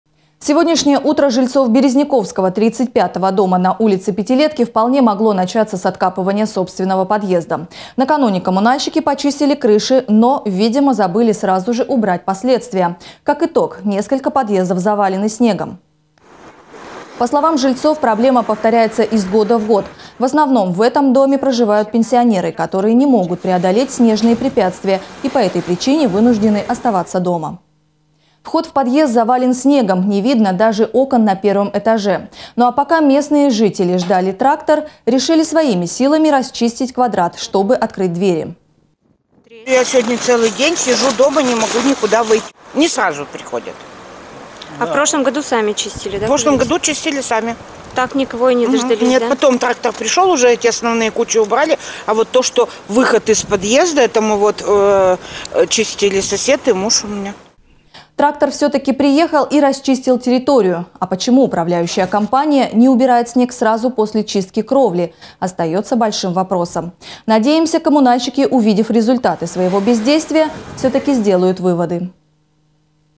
Подъезд в Березниках завалило снегом
Накануне жильцов березниковского 35-го дома на улице Пятилетки вполне могло начаться с откапывания собственного подъезда. Накануне коммунальщики почистили крыши, но, видимо, забыли сразу же убрать последствия. Как итог - несколько подъездов завалены снегом.